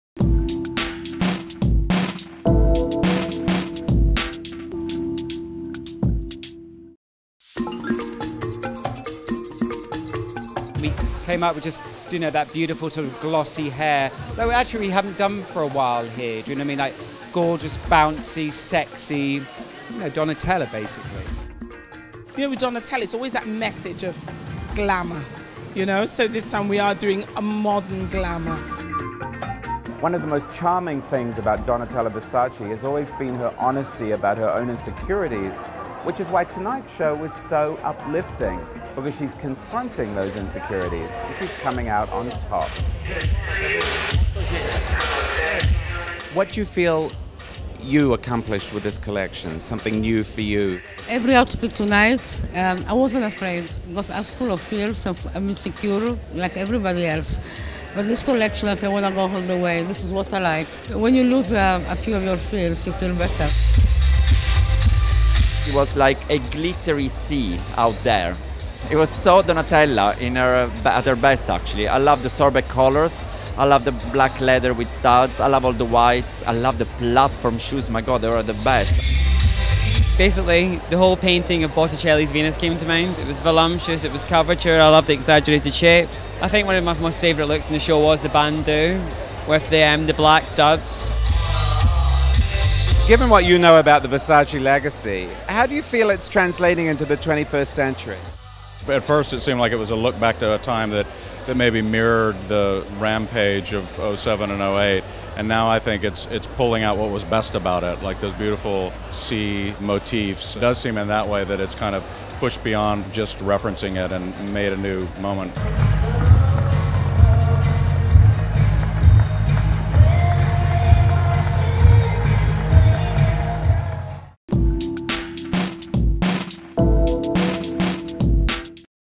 Fashion Show - Versace: Spring 2012 Ready-to-Wear
Runway, backstage, and front-row footage from the Milan show.  Visit Style.com for more runway shows, fashion trends, shopping guides, and news about models and designers.

Starring: Donatella Versace